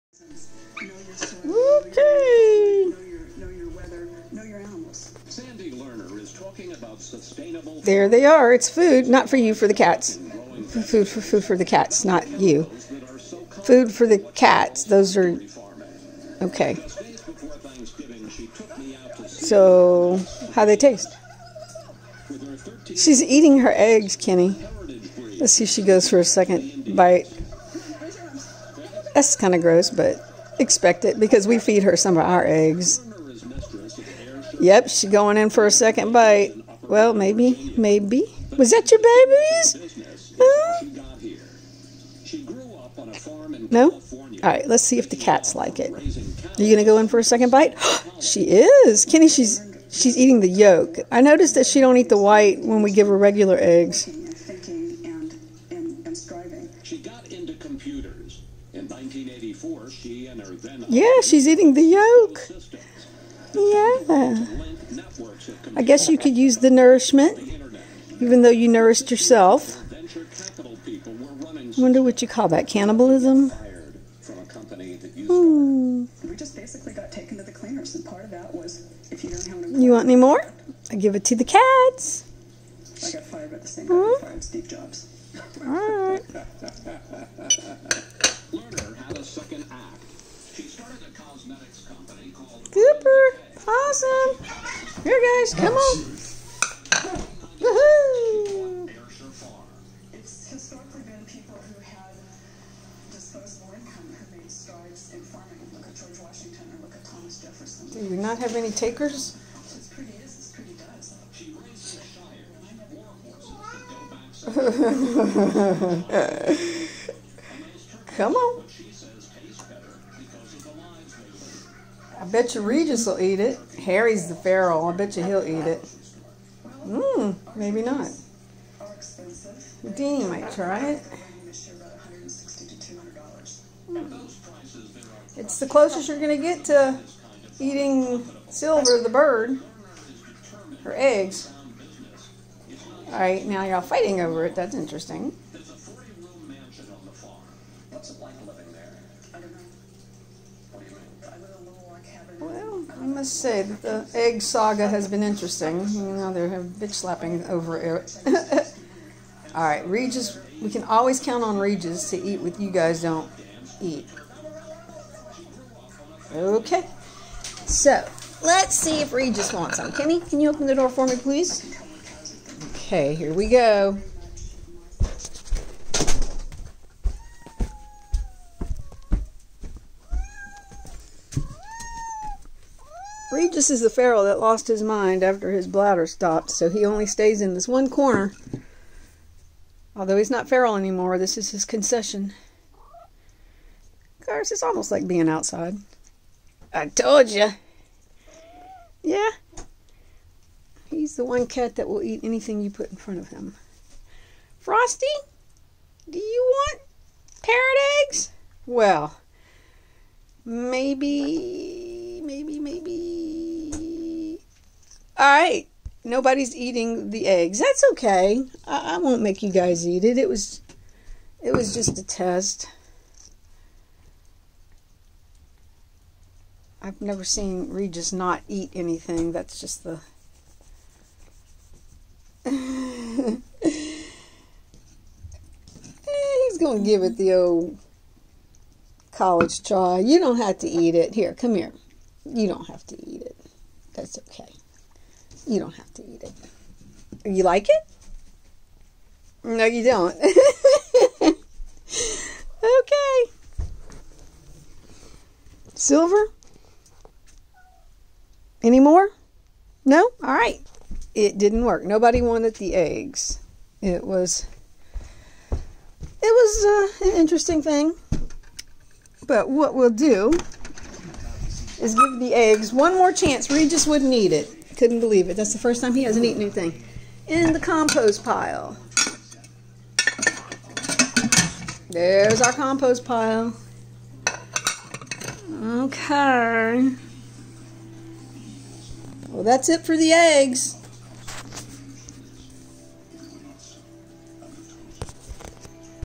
Silver Parrot Eggs Fried Fail Ken November 29 2020 Houdini Harry Cleo Regis Rascal Frosty
related video links in this description.
birds in captivity still lay eggs and when they nest like she enjoyed being in that drawer it helps to produce the environment of brittle bones and her leg broke when she was in season and an experience that overdosed her and she never woke... when we got the call to pick her up not alive... My first thought was did she know she died? 22 years of loving I'll never get over her loss but as you read this I know there are people you've lost close to you I just don't know how to get over a 22-year-old 3-year-old I cared for... And this was just a bird.
trying for a fourth egg

download.